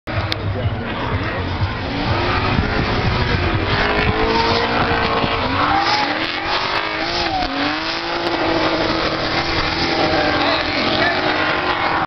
drift poznań - mistrzostwa polski - tor poznań 2013
nie ma to jak zapach palonej gumy o poranku - zobacz drifterów w akcji - top 16 - niedziela na torze poznań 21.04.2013